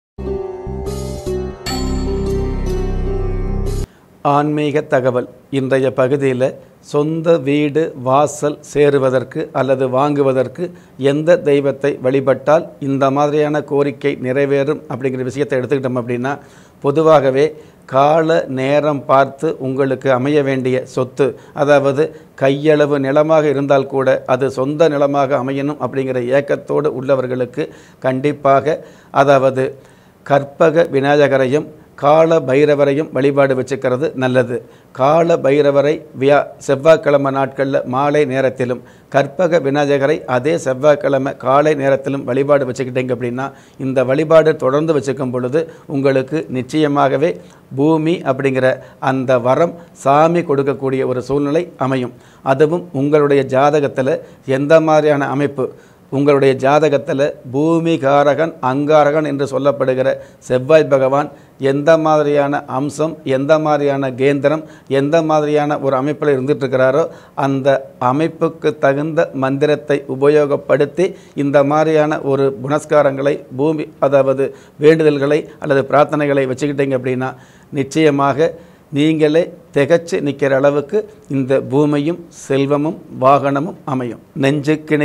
சொந்த , வீடு , நிலம் வாங்க இந்த இந்த தெய்வத்தை வணங்குங்கள் | ஆன்மீக தகவல்
சொந்த , வீடு , நிலம் வாங்க இந்த இந்த தெய்வத்தை வணங்குங்கள் | ஆன்மீக தகவல் | Aanmeega Thagaval | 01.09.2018